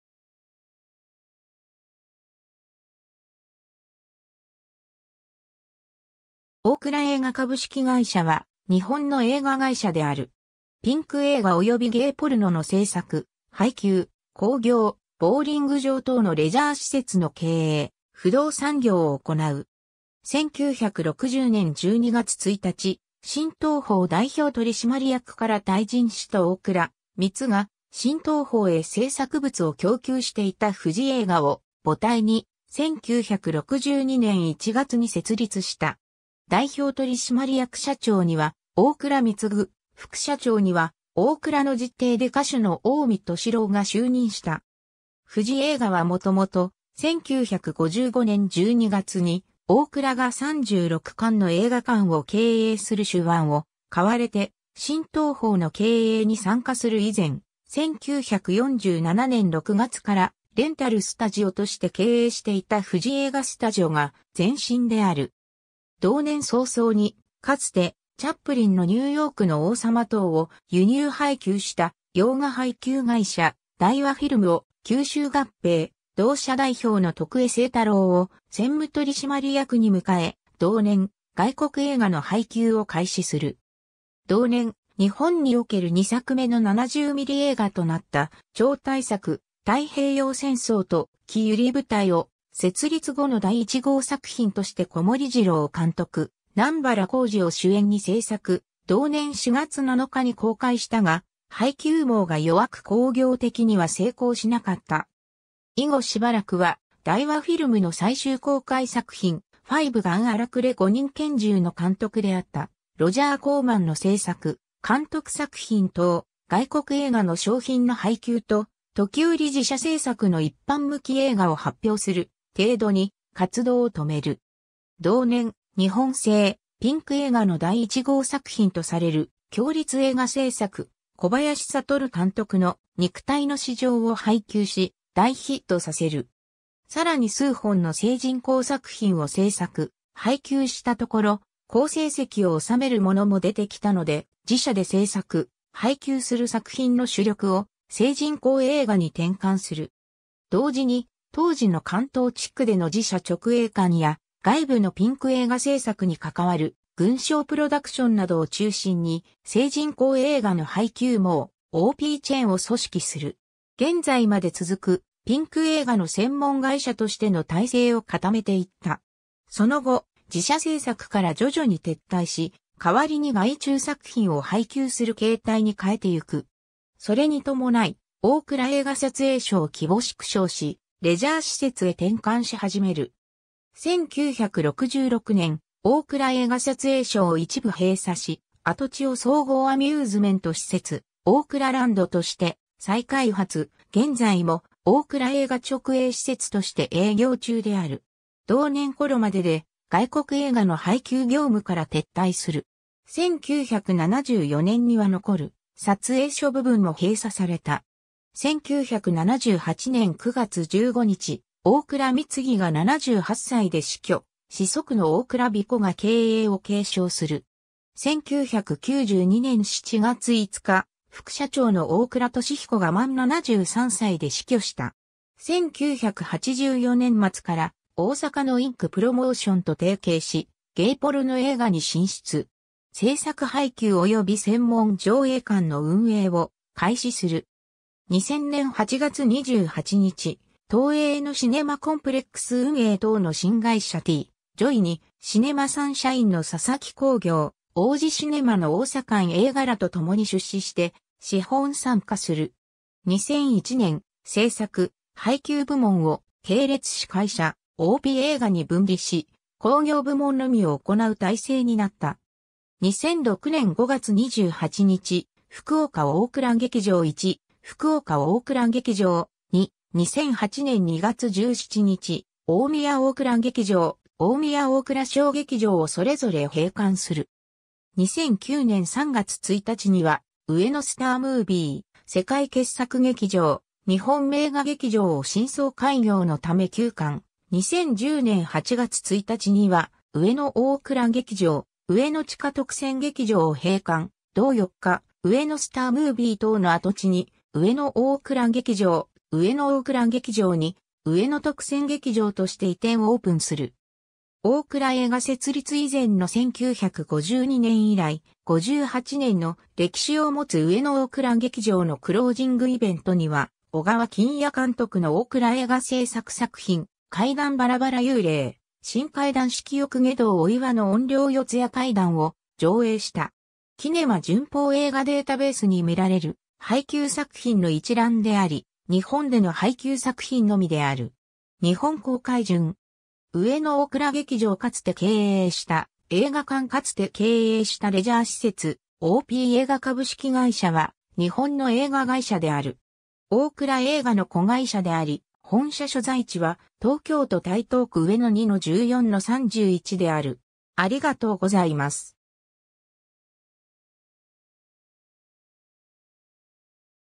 大蔵映画
ゲイ・ポルノ
大蔵映画株式会社（おおくらえいが-）は、日本の映画会社である。
ピンク映画及びゲイ・ポルノの製作・配給・興行、ボウリング場等のレジャー施設の経営、不動産業を行う。
 1960年（昭和35年）12月1日、新東宝代表取締役から退陣した大蔵貢が、新東宝へ製作物を供給していた富士映画を母体に、1962年（昭和37年）1月に設立した。
代表取締役社長には大蔵貢、副社長には大蔵の実弟で歌手の近江俊郎（大蔵敏彦）が就任した。
富士映画はもともと、1955年（昭和30年）12月に大蔵が36館の映画館を経営する手腕を買われて、新東宝の経営に参加する以前、1947年（昭和22年）6月からレンタルスタジオとして経営していた富士映画スタジオが前身である。
同年早々に、かつて『チャップリンのニューヨークの王様』等を輸入配給した洋画配給会社大和フィルムを吸収合併、同社代表の徳江清太郎を専務取締役に迎え、同年、外国映画の配給を開始する。
 同年、日本における2作目の70ミリ映画となった超大作『太平洋戦争と姫ゆり部隊』を、設立後の第1号作品として小森白を監督、南原宏治を主演に製作、同年4月7日に公開したが、配給網が弱く興行的には成功しなかった。
以後しばらくは、大和フィルムの最終公開作品『ファイブ・ガン あらくれ5人拳銃』の監督であったロジャー・コーマンの製作・監督作品等、外国映画の小品の配給と、時折自社製作の一般向映画を発表する程度に、活動を留める。
 同年、日本製ピンク映画の第1号作品とされる協立映画製作、小林悟監督の『肉体の市場』を配給し大ヒットさせる。
さらに数本の成人向作品を製作・配給したところ、好成績を収めるものも出てきたので、自社で製作・配給する作品の主力を、成人向映画に転換する。
同時に、当時の関東地区での自社直営館や、外部のピンク映画製作に関わる群小プロダクションなどを中心に、成人向映画の配給網「OPチェーン」を組織する。
現在まで続く、ピンク映画の専門会社としての体制を固めていった。
その後、自社製作から徐々に撤退し、代わりに外注作品を配給する形態に変えていく。
それに伴い、大蔵映画撮影所を規模縮小し、レジャー施設へ転換し始める。
 1966年（昭和41年）、大蔵映画撮影所を一部閉鎖し、跡地を総合アミューズメント施設「オークラランド」として再開発、現在も、大蔵映画直営施設として営業中である。
同年ころまでで外国映画の配給業務から撤退する。
1974年には残る撮影所部分も閉鎖された。
 1978年（昭和53年）9月15日、大蔵貢が78歳で死去、子息の大蔵满彦が経営を継承する。
1992年（平成4年）7月5日、副社長の大蔵敏彦（近江俊郎）が満73歳で死去した。
 1984年（昭和59年）末から、大阪のENKプロモーション（東梅田日活株式会社）と提携し、ゲイポルノ映画に進出。
製作配給及び専門上映館の運営を開始する。
 2000年（平成12年）8月28日、東映のシネマコンプレックス運営等の新会社ティ・ジョイに、シネマサンシャインの佐々木興業、王子シネマの大旺映画らとともに出資して資本参加する。
 2001年（平成13年）、製作・配給部門を系列子会社・オーピー映画に分離し、興行部門のみを行う体制になった。
 2006年（平成18年）5月28日、福岡オークラ劇場Ⅰ・福岡オークラ劇場Ⅱ、2008年（平成20年）2月17日、大宮オークラ劇場・大宮オークラ小劇場をそれぞれ閉館する。
2009年（平成21年）3月1日には、上野スタームービー、世界傑作劇場、日本名画劇場を新装開業のため休館、2010年（平成22年）8月1日には上野オークラ劇場、上野地下特選劇場を閉館、同4日、上野スタームービー等の跡地に上野オークラ劇場、上野オークラ劇場2、上野特選劇場として移転オープンする。
大蔵映画設立以前の1952年（昭和27年）以来、58年の歴史を持つ上野オークラ劇場のクロージングイヴェントには、小川欽也監督の大蔵映画製作作品『怪談バラバラ幽霊』（1968年）、『新怪談色欲外道 お岩の怨霊四谷怪談』（1976年）を上映した。
 キネマ旬報映画データベースに見られる配給作品の一覧であり、日本での配給作品のみである。
日本公開順。
 上野オークラ劇場（2013年6月9日撮影） かつて経営した映画館 かつて経営したレジャー施設 オーピー映画株式会社（オーピーえいが-）は、日本...